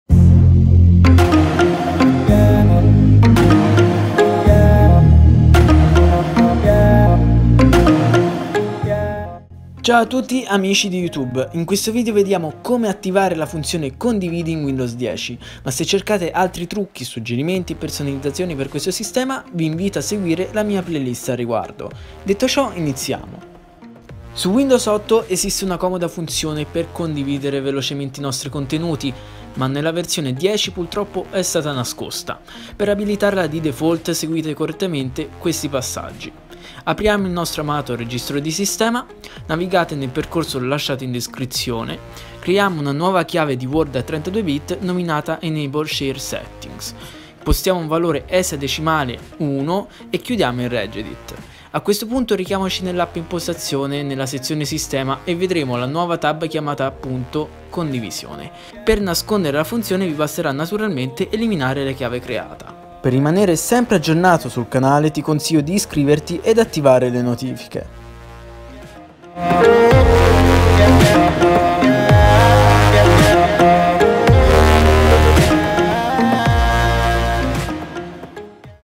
Attivare la funzione Condividi in Windows 10 Anniversary Update
*COMPENDIO*
Su Windows 8 esiste una comoda funzione per condividere velocemente dei contenuti, ma purtroppo nella versione 10 tale chicca è stata nascosta.

Per riabilitarla seguire questi passaggi.
1] Aprire il Registro di sistema (comando 'regedit' nella finestra Esegui).
2] Recarsi in HKEY_CURRENT_USER/Control Panel.
3] Creare una chiave DWORD a 32 bit, chiamandola "EnableShareSettings".
4] Impostare come valore esadecimale 1 e chiudere il Regedit.
5] Aprire l’app Impostazioni, andare su Sistema e verrà visualizzata la nuova voce "Condivisione".

Ora sarà possibile visualizzare la nuova funzione Condivisione all’interno della scheda Condividi dell’Esplora file di Windows. Per nasconderla, basterà semplicemente eliminare la chiave precedentemente creata.

Nota
• La guida è valida per Windows 10 Anniversary Update (build 1607), colle versioni successive Microsoft ha ripristinato in parte questa funzionalità.
• Non mi ritengo responsabile di danni hardware e software causati da questa o dalle altre guide pubblicate su questo canale YouTube. Il tutto è fatto a vostro rischio e pericolo e potrebbe comportare, nel peggiore dei casi, la perdita dei dati presenti nel dispositivo: prima di prendere qualsiasi iniziativa, consiglio vivamente di effettuare un backup completo.

*DATI VIDEO*
• Programma di registrazione e montaggio: Wondershare Filmora 9